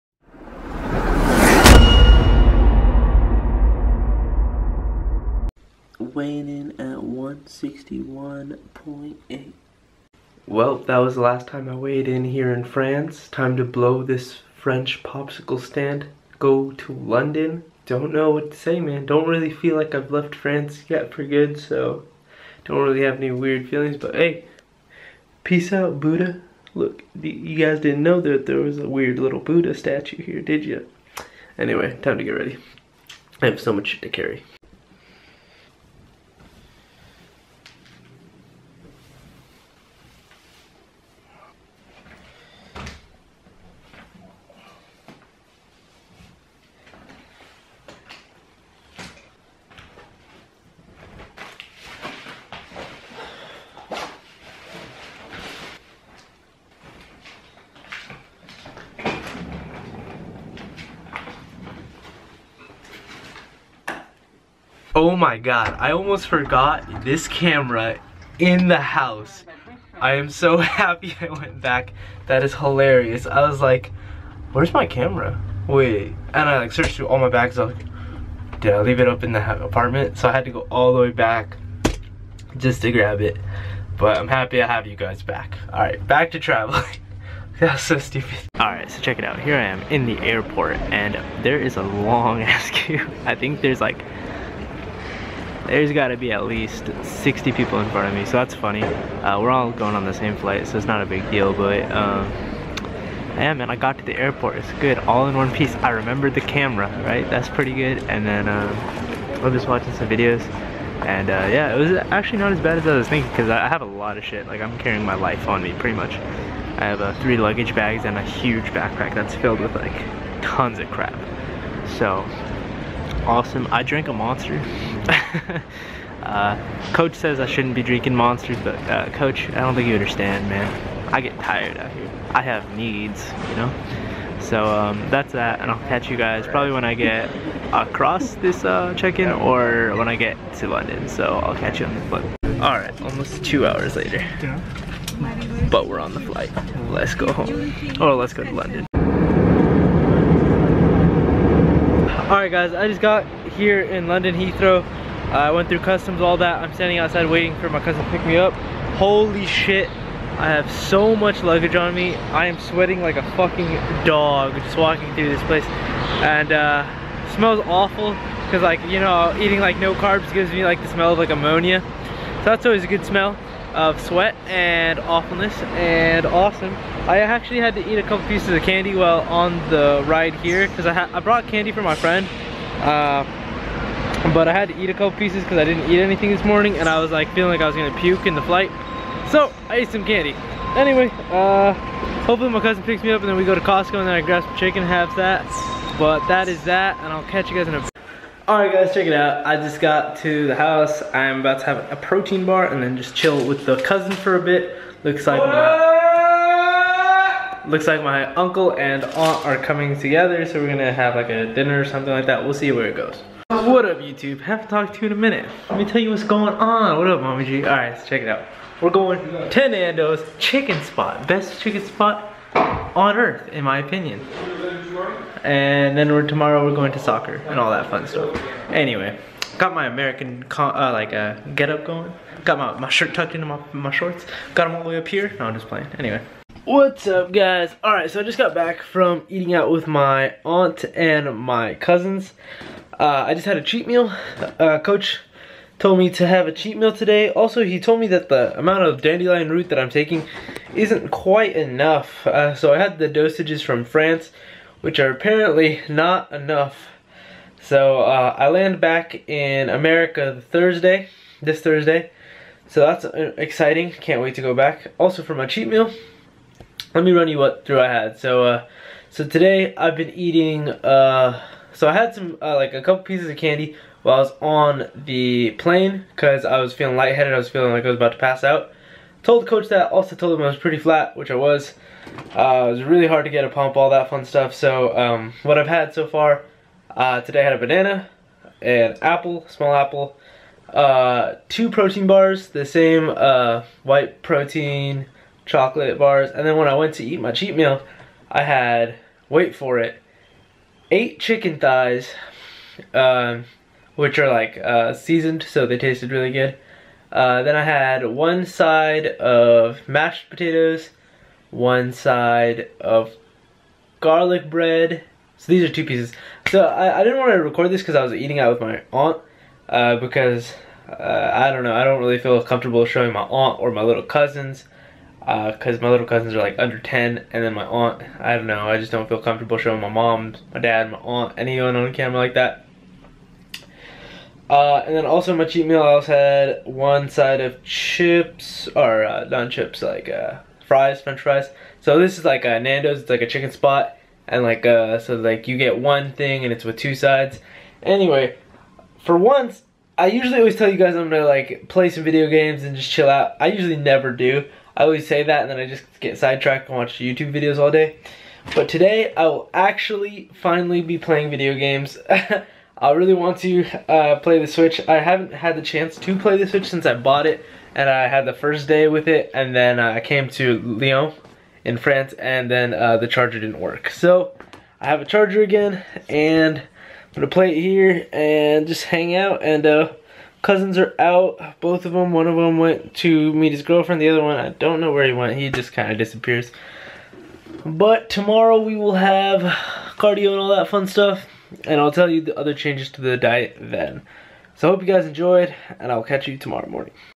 Back Home | Summer Shredding 75
Making a quick stop by the cousins before a trip back

Looking to improve my life, and I believe recording my progress will help me do that. Today I ran through what this series is all about, constant improvement. I'm positive I will improve with time, and it will be fun to watch.